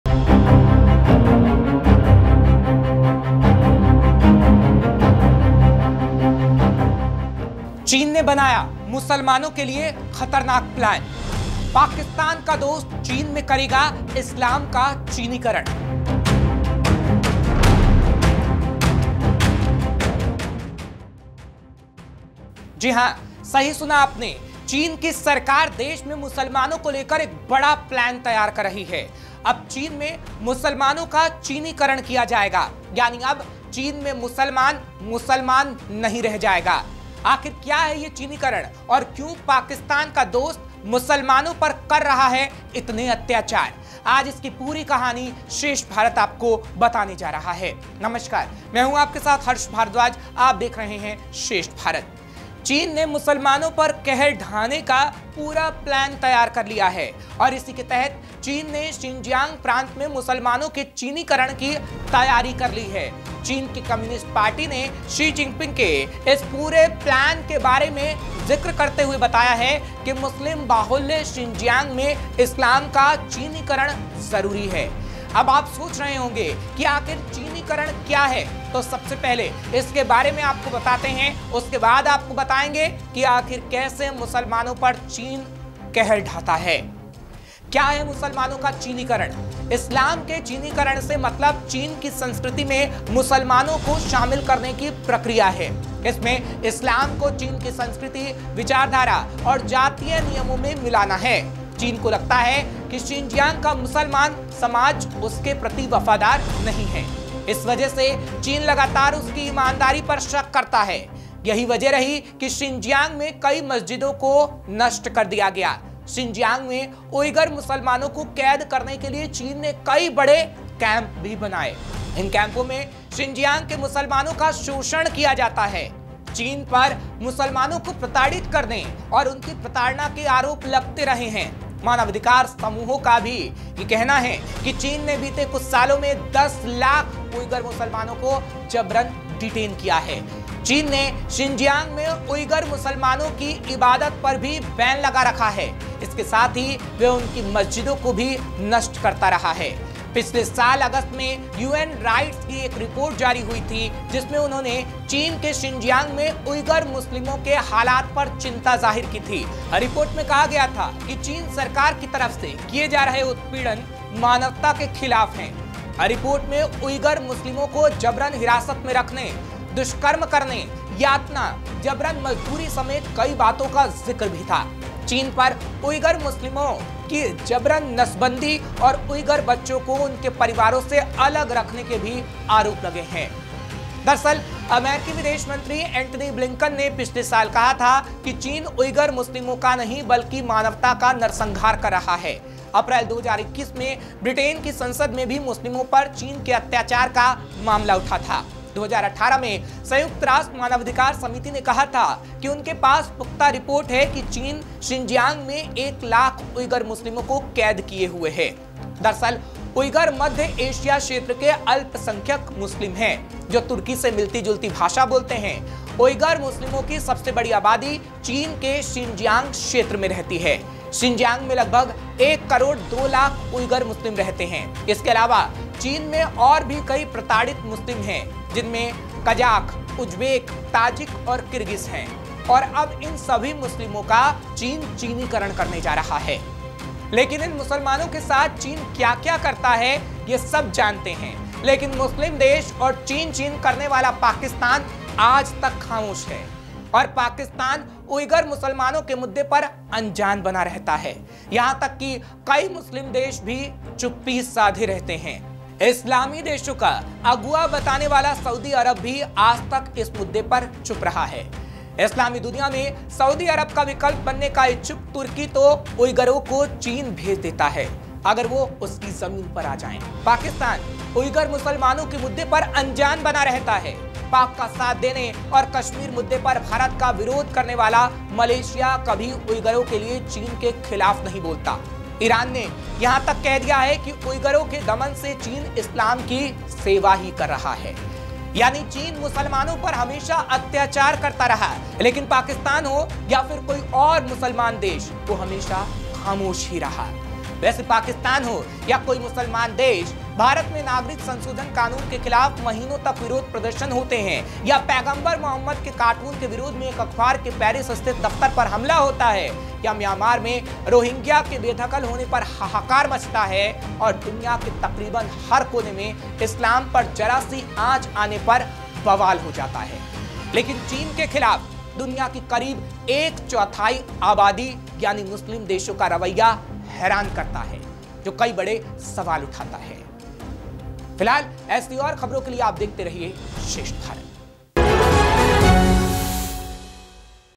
China ने बनाया Muslims के लिए खतरनाक प्लान, मुसलमानों का चीनीकरण करेगा China
चीन (china) ने मुसलमानों पर कहर ढहाने का पूरा प्लान तैयार कर लिया है और इसी के तहत चीन (china) के शिनजियांग प्रांत में मुसलमानों (muslims) के चीनीकरण की तैयारी हो रही है. चीन (china) की कम्युनिस्ट पार्टी (communist party) ने शी जिनपिंग (xi jinping) के इस पूरे प्लान के बारे में जिक्र करते हुए कहा है कि,,,मुस्लिम बाहुल्य शिनजियांग में इस्लाम का चीनीकरण जरूरी है